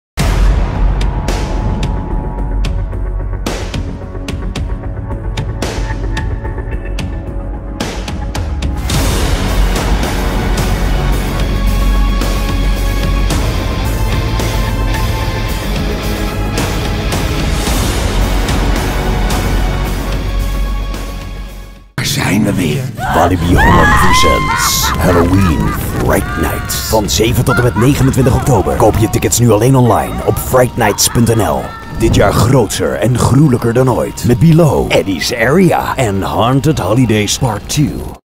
Halloween Fright Nights 2017 (Evil Is Here...) - Quarantine
When there's no more room in hell, the dead will walk the earth... Wat doe jij als je oog in oog staat met een bloeddorstige zombie tijdens Halloween? Stopt je hart met kloppen, verstijf je of zet je het op het lopen?

Tijdens Halloween Fright Nights verzamelen rottende zombies, duivelse demonen en bloederige Rednecks zich in Walibi Holland. Met 7 intense Scare Zones, 6 bloedstollende Haunted Houses, een heftige walktrough en een vette horrorshow wordt oktober gruwelijker dan ooit.
Evil is here…